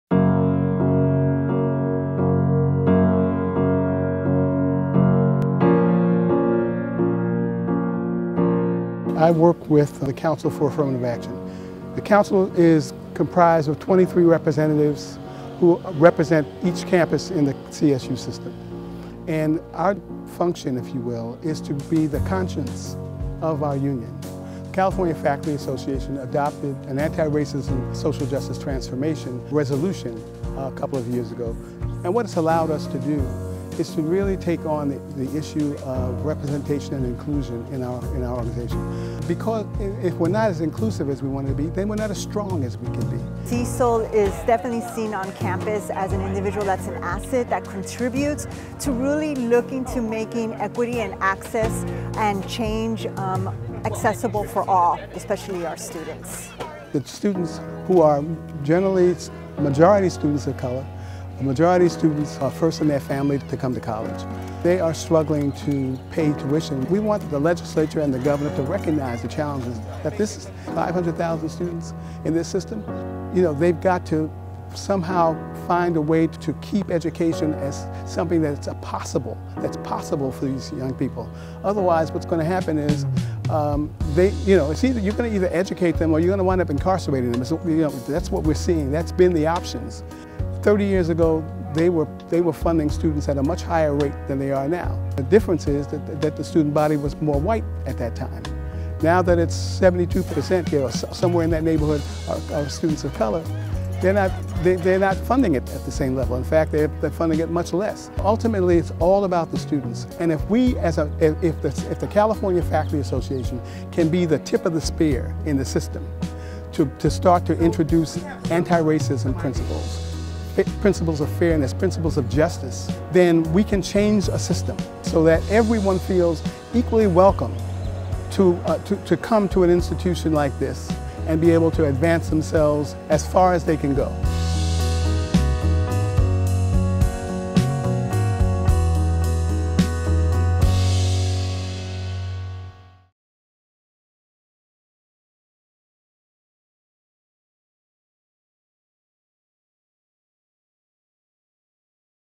H. Councill Trenholm Memorial Award (Black) – Dr. Cecil Canton (CA)
NEA bestows upon Dr. Cecil Canton its highest honor for work to advance racial and social justice